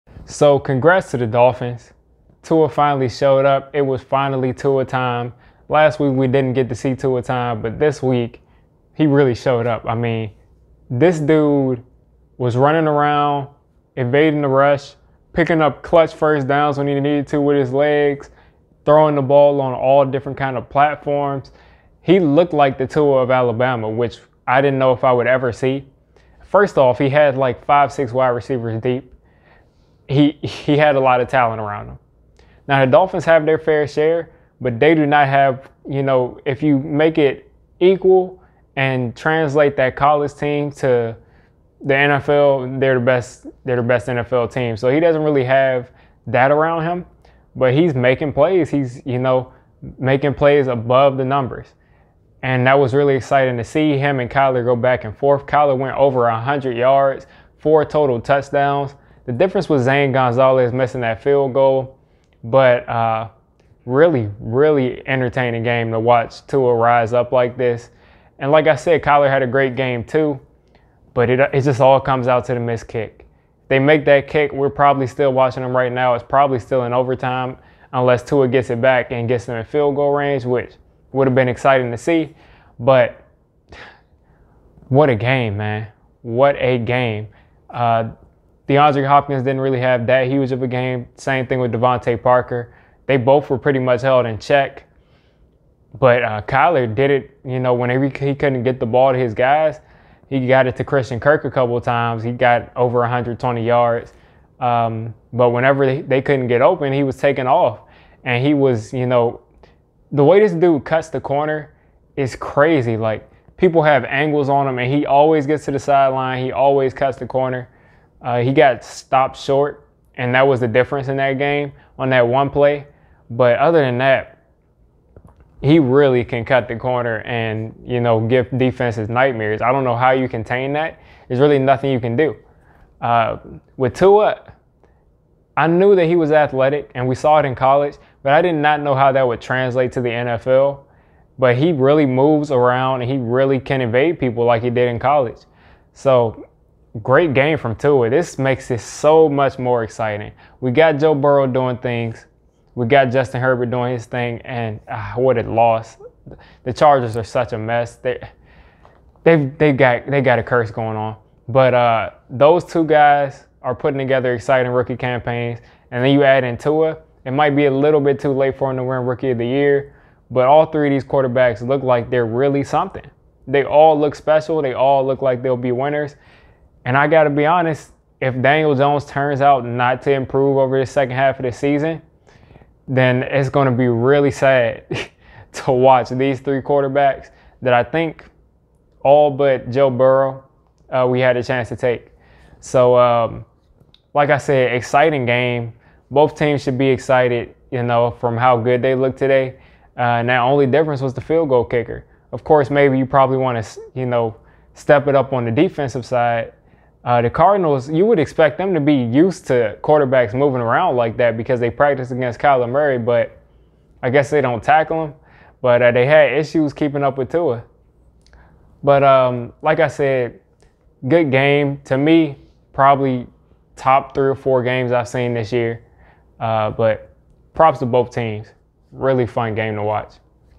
Tua Tagovailoa vs Kyler Murray | Most Exciting Game of the Year!
Miami Dolphins QB Tua Tagovailoa got his second win and looked great against Kyler Murray and the Cardinals. This game showed both QBs elusiveness and mobility and turned into a shootout. Zane Gonzalez ended up missing the field goal that clinched the win for the Miami Dolphins.